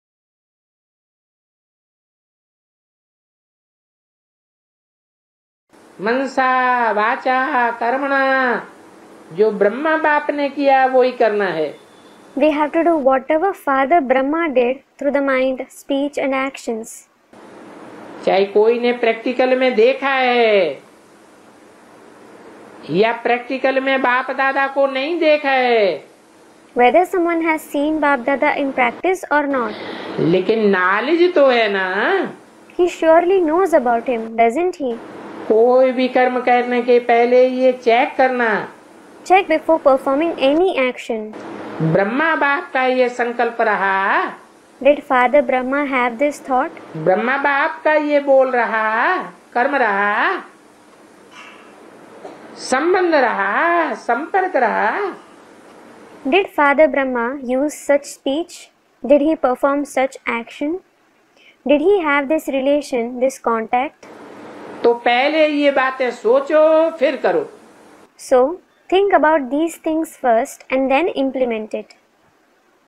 1254 Follow the mother in actions vcd1563
1254 Follow the mother in actions vcd1563

We have to do whatever father Brahma did through the mind, speech and actions. Whether someone has seen Bapdada in practice or not, he surely knows about him, doesn’t he? Check before performing any action: did father Brahma have this thought? Did father Brahma use [such] speech? Did he perform such action, did he have this relation, this contact? So, think about these things first and then implement it.

Copyright :    AIVV